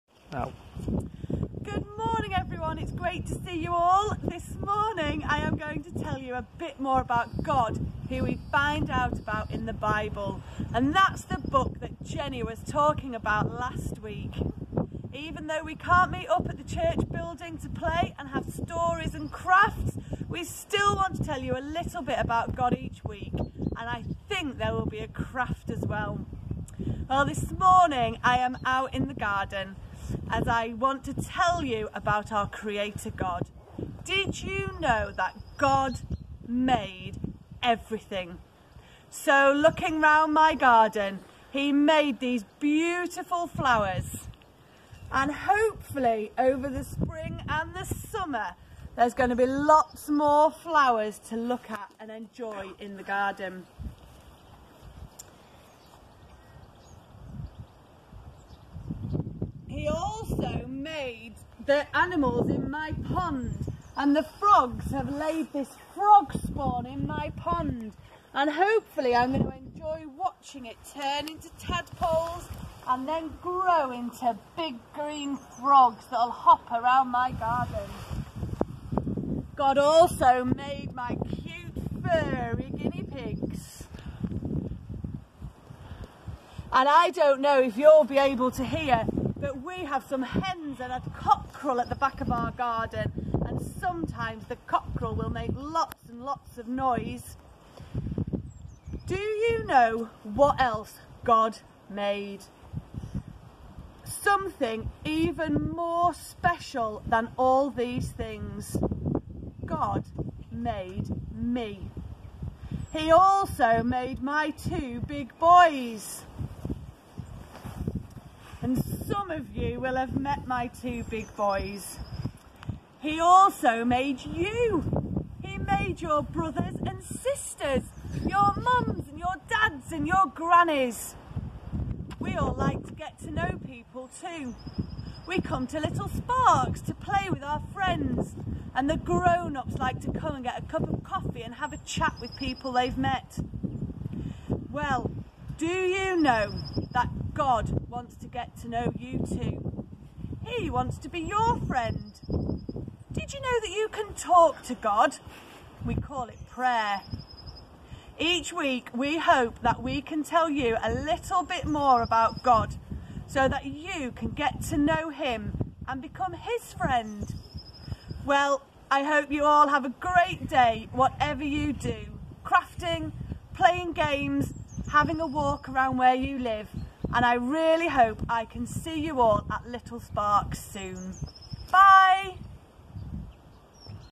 Week 4 - God created everything and he wants to know you!
Little Sparks Toddler Group Story - Week 4!

For more information about Little Sparks Toddler Group, visit the Facebook

Another video brought to you for free by Carlisle Baptist Church, Cumbria, UK